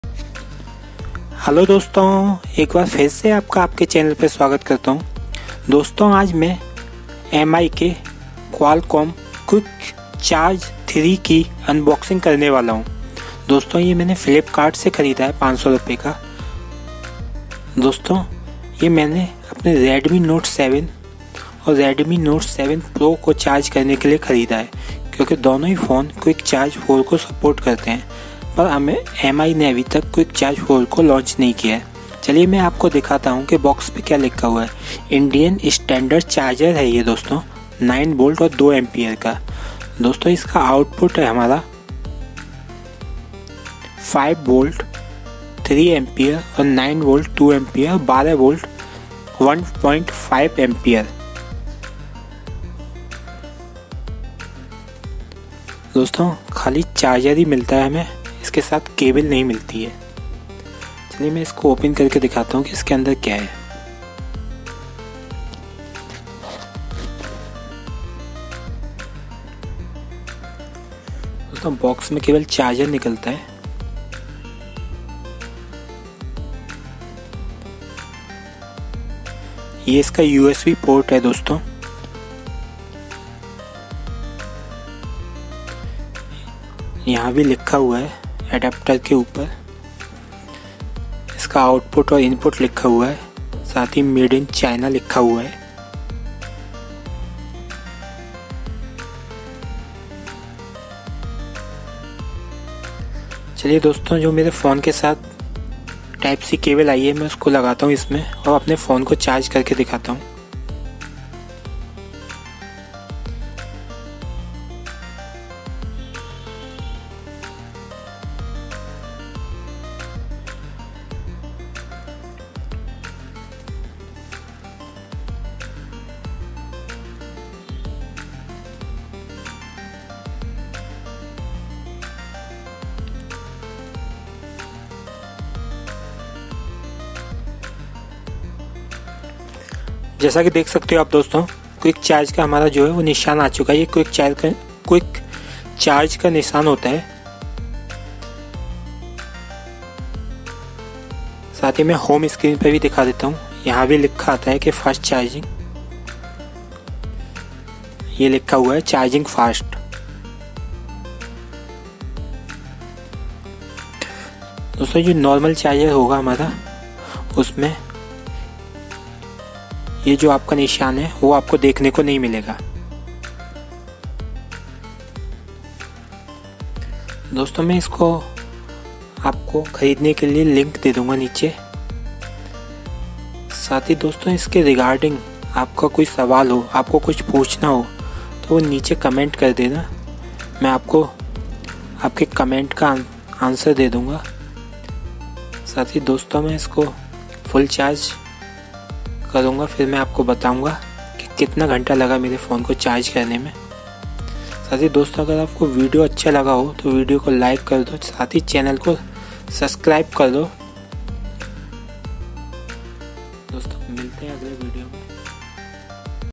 Xiaomi Fast Charger For Redmi Note 7, 7Pro Supports Qualcomm Quick Charge 3
Dosto aaj me mi ke Qualcomm quick charge 3 ki unboxing karne wala hu.
Ye charger aapke Redmi Note 7 aur Redmi Note 7 Pro ke liye aacha rehga.

Thanks,
Milan Raghav